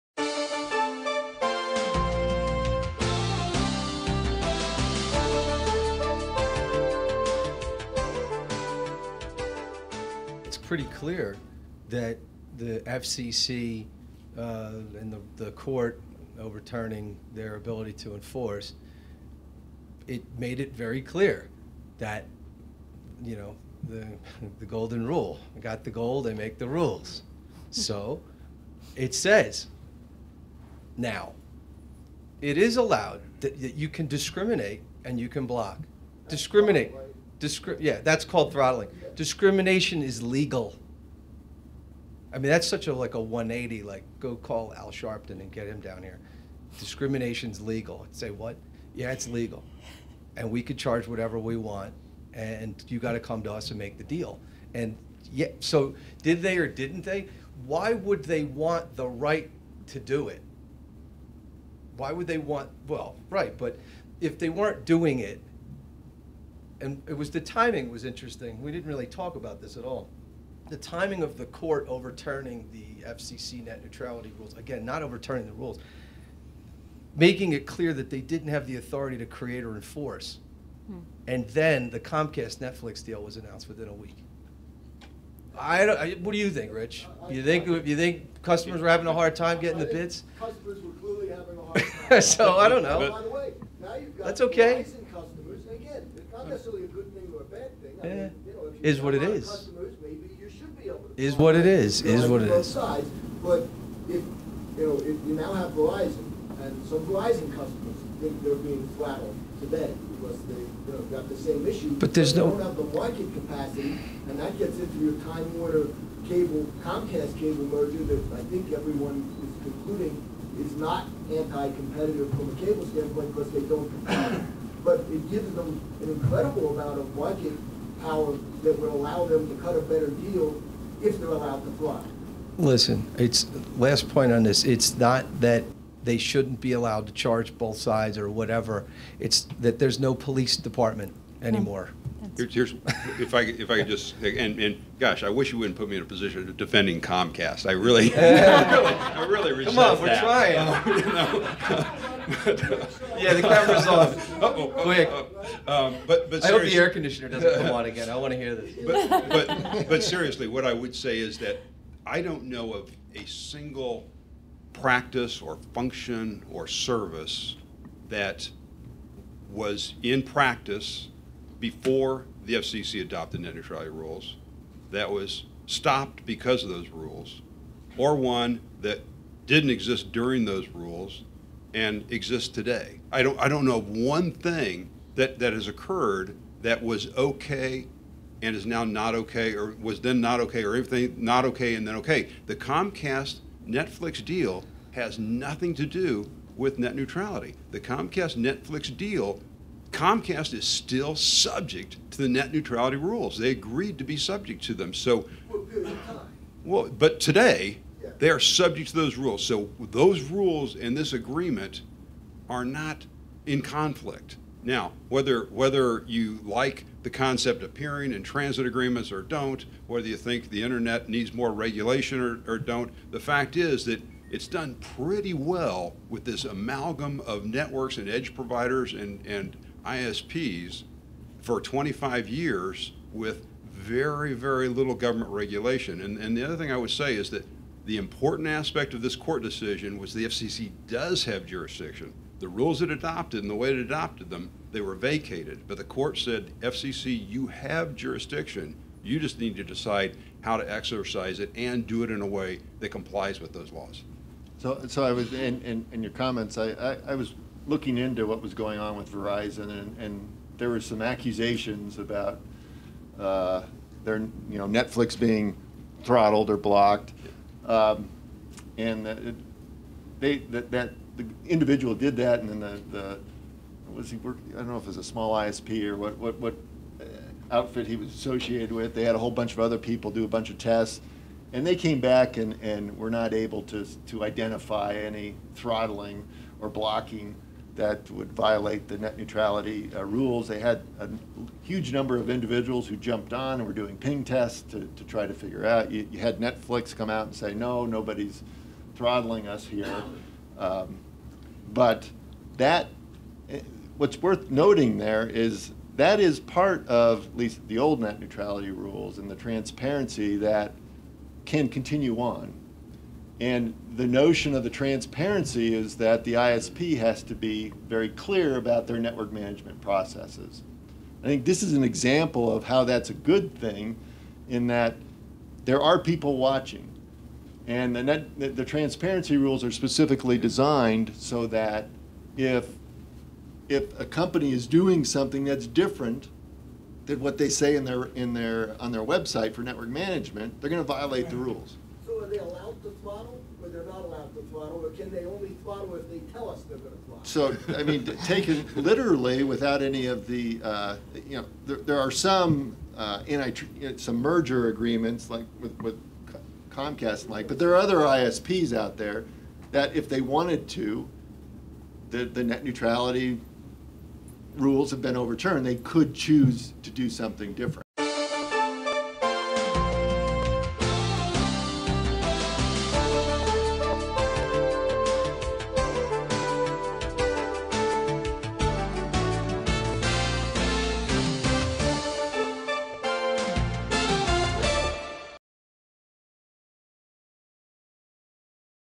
Last Mile, Throttling, and Net Neutrality
Panelists on the CEO Roundtable "The Net Neutrality Ruling: Unchartered Territory" at Telecom Exchange West at Sports Authority Field at Mile High on March 6, 2014 discuss the FCC implications of section 706 and common carrier regulation.